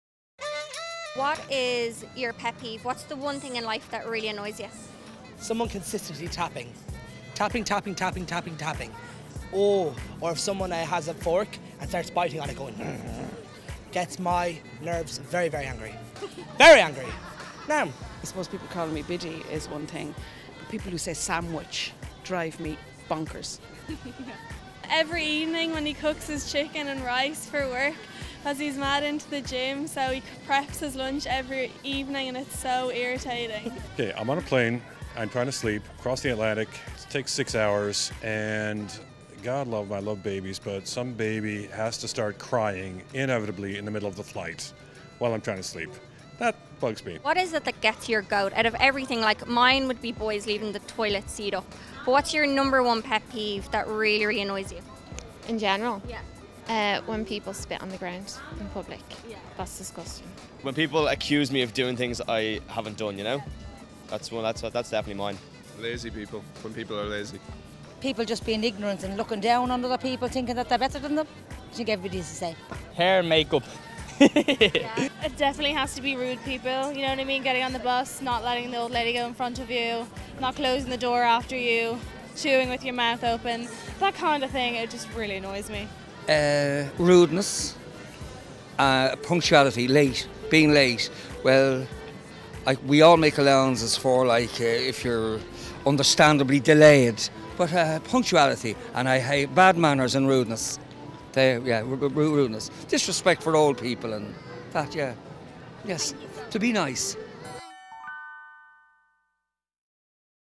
Watch! What grinds your gears?
What are your biggest pet peeves? Laura Delaney finds out what really gets on your nerves.